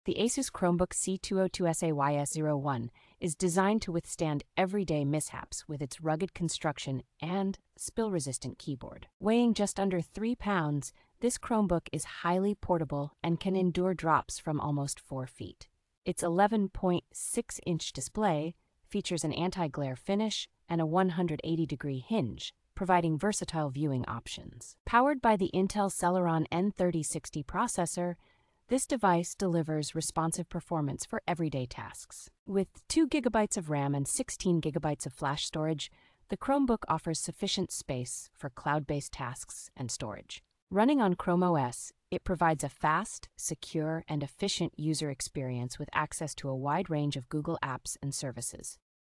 REVIEW (2024): Asus Chromebook C202SA-YS01 11.6". ESSENTIAL details.
The Asus Chromebook C202SA-YS01 is designed to withstand everyday mishaps with its rugged construction and spill-resistant keyboard.

Weighing just under three pounds, this Chromebook is highly portable and can endure drops from almost four feet.

Its eleven point six inch display features an anti-glare finish and a one hundred eighty degree hinge, providing versatile viewing options.

Powered by the Intel Celeron N3060 processor, this device delivers responsive performance for everyday tasks.

With two gigabytes of RAM and sixteen gigabytes of flash storage, the Chromebook offers sufficient space for cloud-based tasks and storage.

Running on Chrome OS, it provides a fast, secure, and efficient user experience with access to a wide range of Google apps and services.